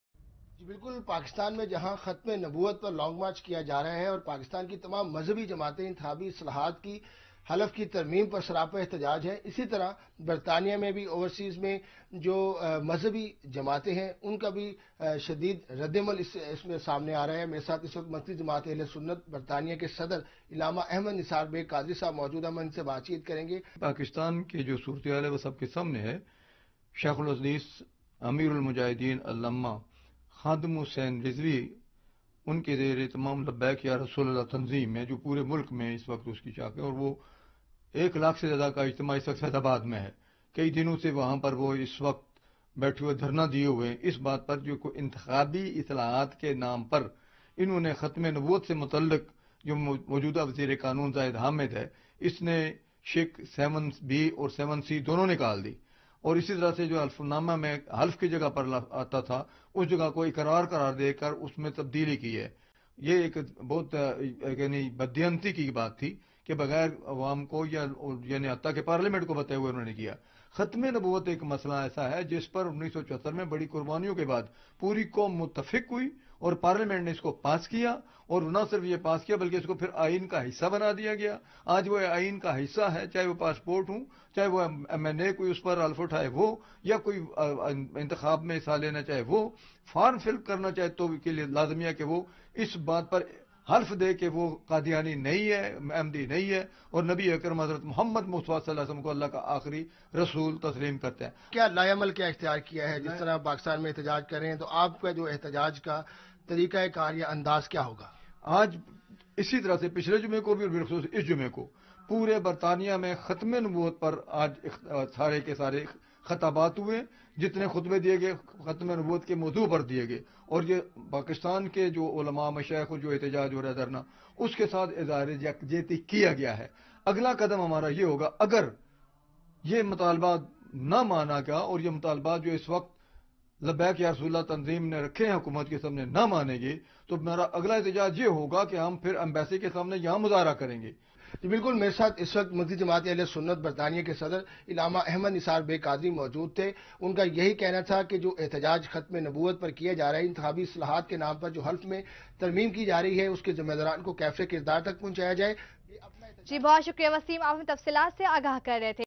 British Muslims express support for Pakistani Islamists against Ahmadis
Rabwah Times